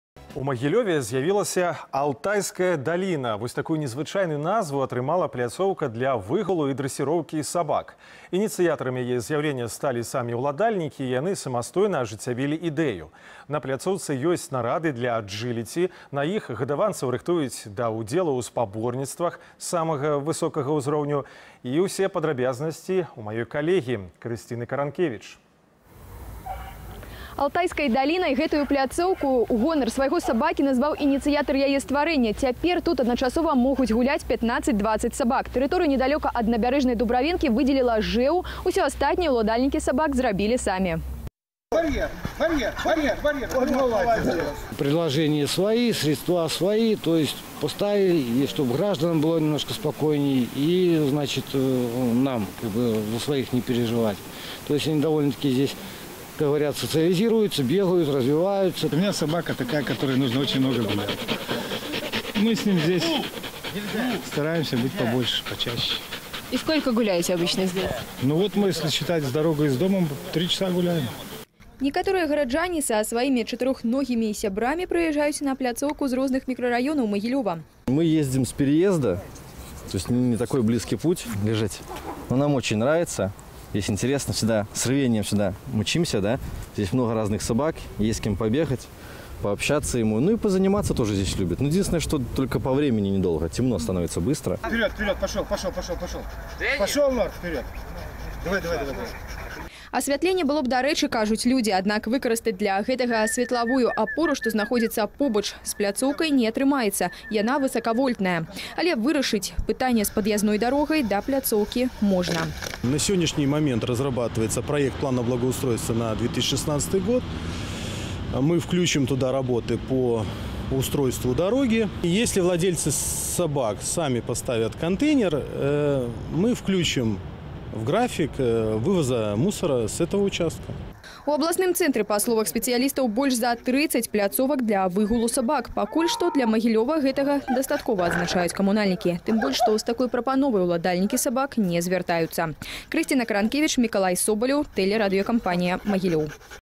Алтайская даліна ў Магілёве. КРАІНА
Такую незвычайную назву атрымала пляцоўка для выгулу і дрэсіроўкі сабак. Ініцыятарамі яе з’яўлення сталі самі ўладальнікі.